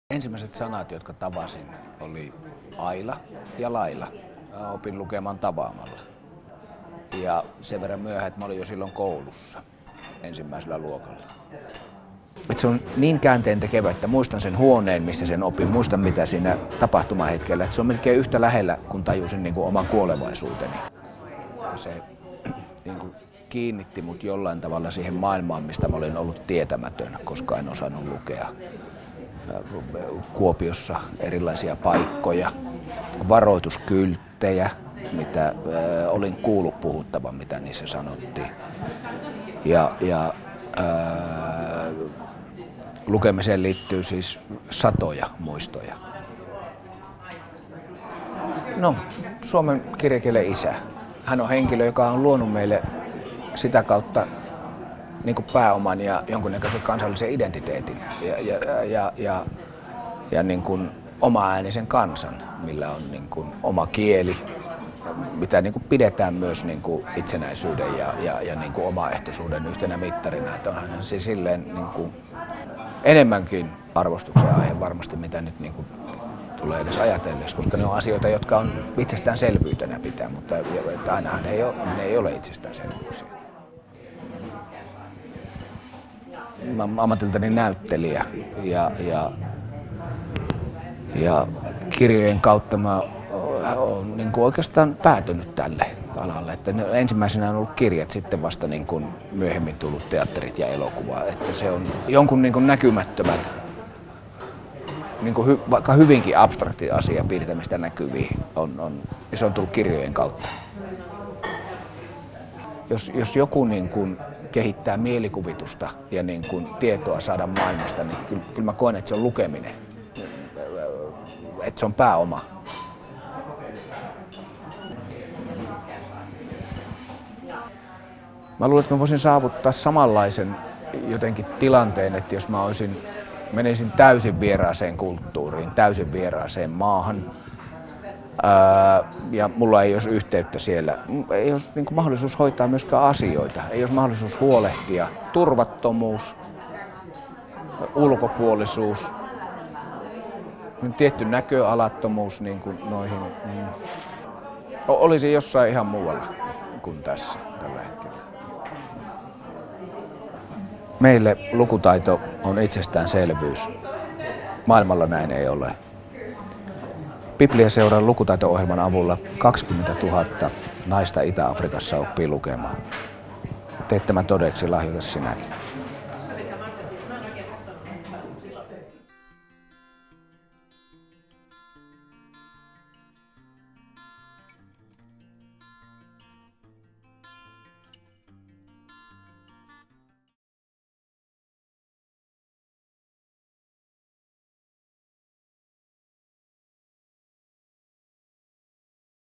Tommi Eronen: Missä sinä olisit ilman Agricolaa?
Agricolan päivää vietetään 9.4. Maailmassa on yli 750 miljoonaa lukutaidotonta aikuista. Heistä 2/3 on naisia. Pipliaseurojen lukutaitotyö antaa mahdollisuuden oppia lukemaan omalla kielellä. Lue lisää: Agricola.fi

Tuotanto: Suomen Pipliaseura